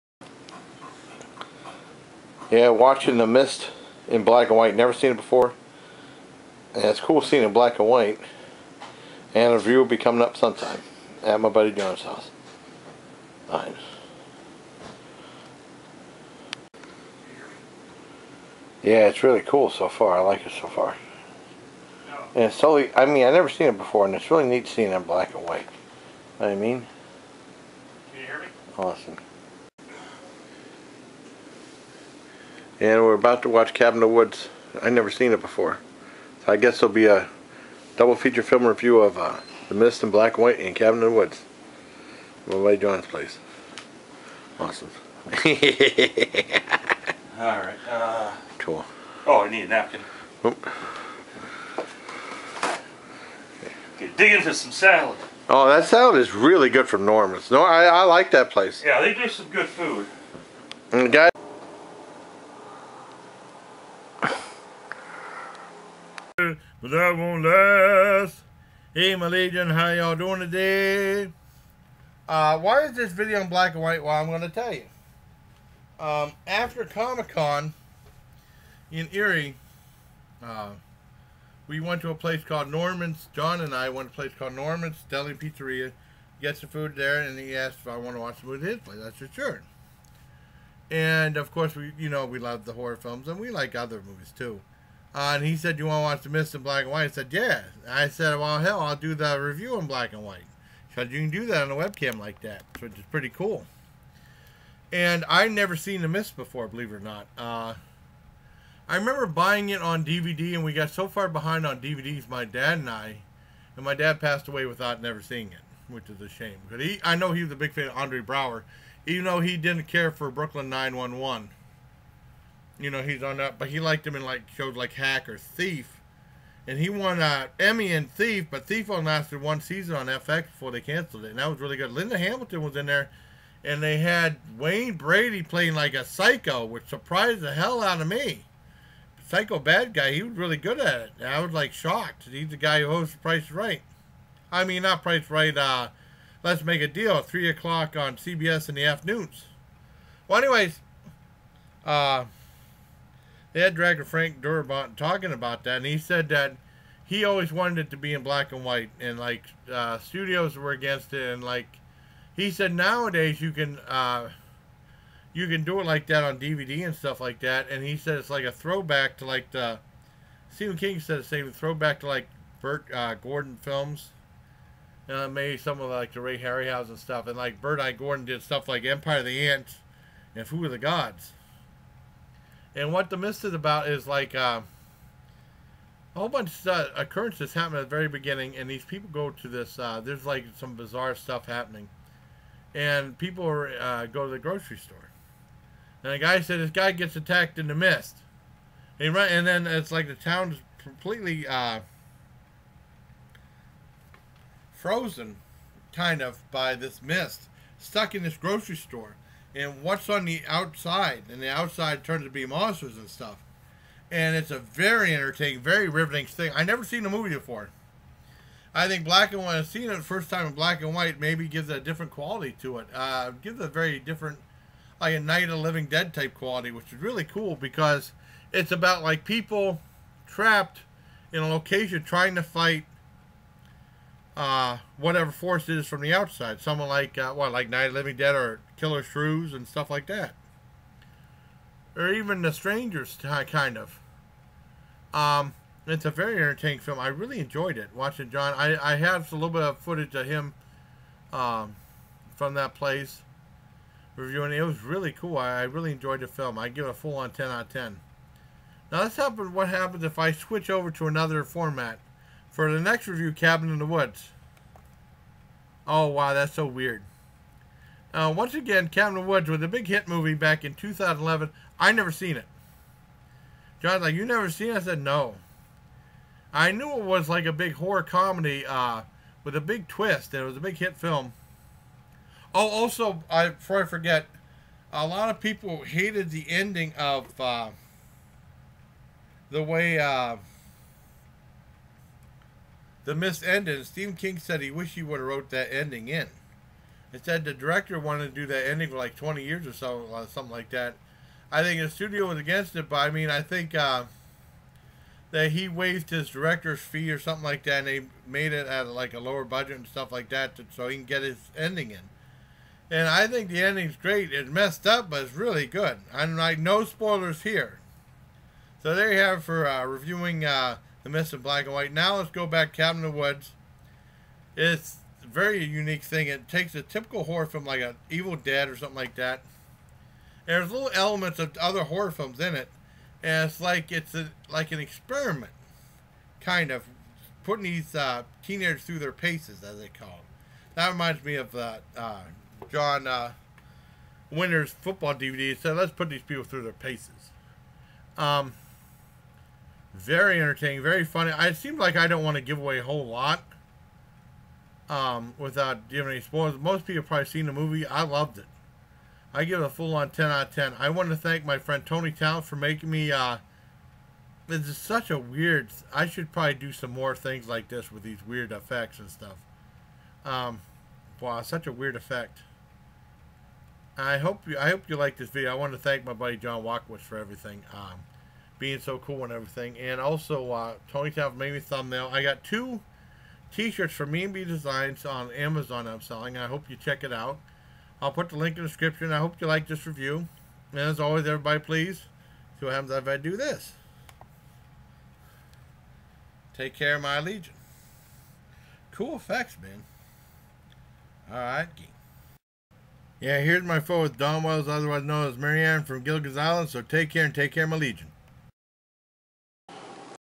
I review THE MIST (in black & white) & CABIN IN THE WOODS
Are these 2 movies any good?